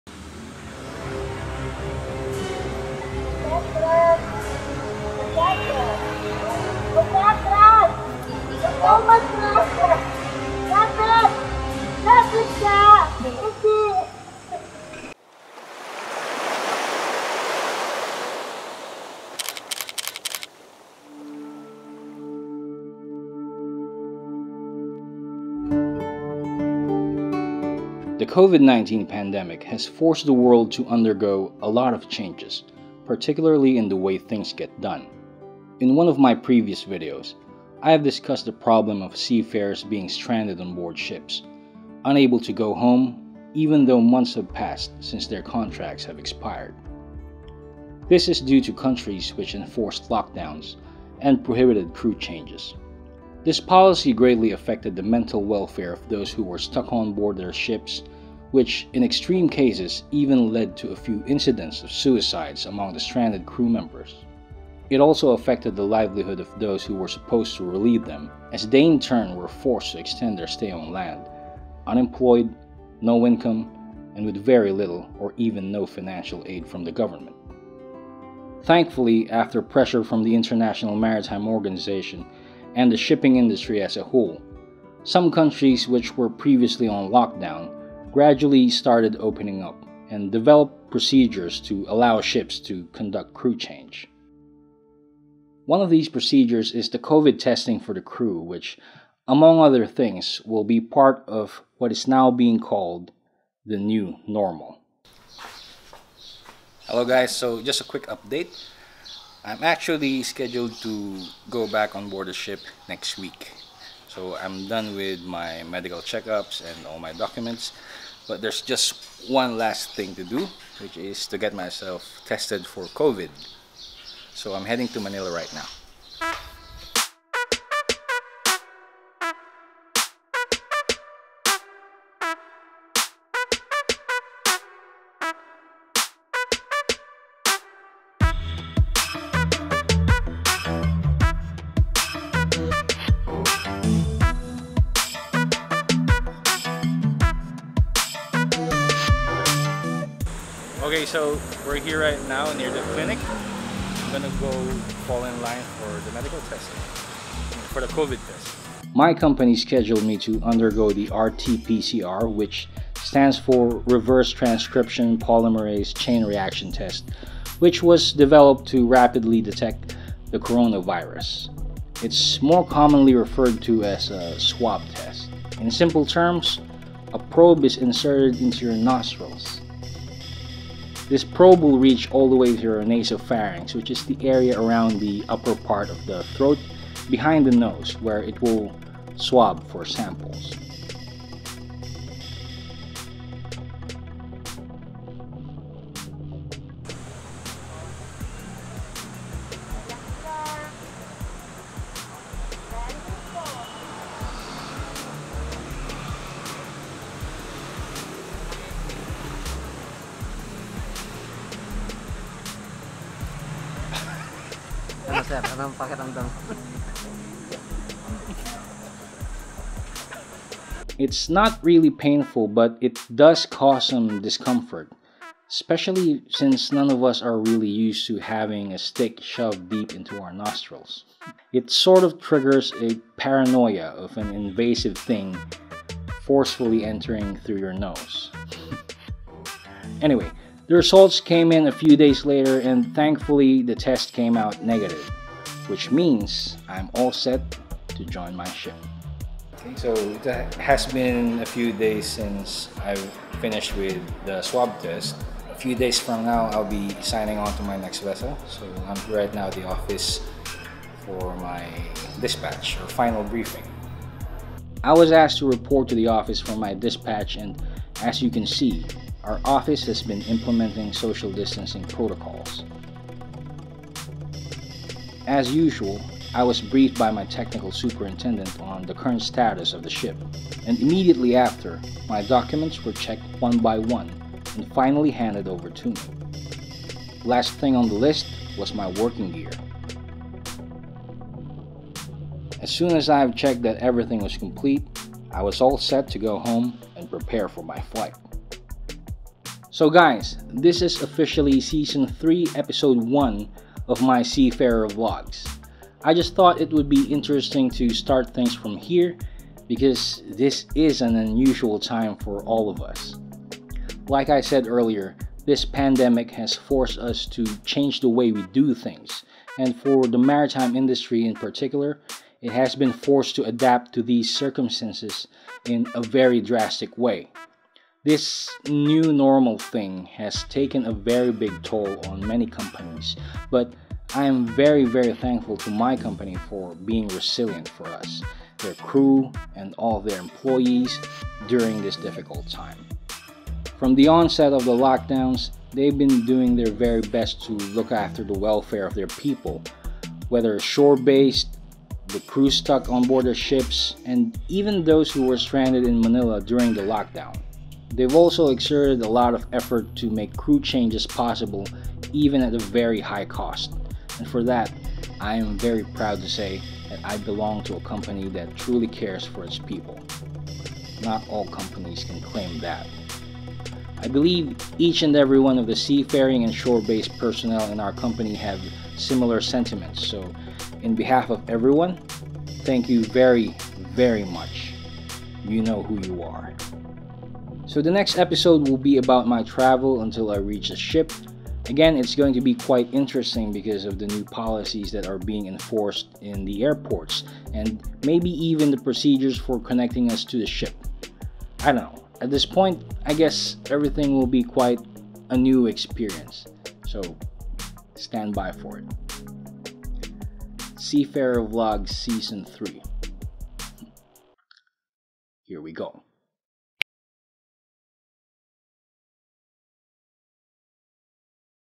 Getting Dispatched To My Next Ship | Chief MAKOi Seaman Vlog S03E01
The New Normal.  A lot of new procedures are being implemented for seafarers going for crew change.  One of the new things being done is COVID Testing.  In this video, I share my experience with the Swab Test and my preparations as I get dispatched to my next ship assignment.

Chief MAKOi
Seaman Vlog
S03E01

Special Thanks to Kuya Maik for the Manila Bay Clips.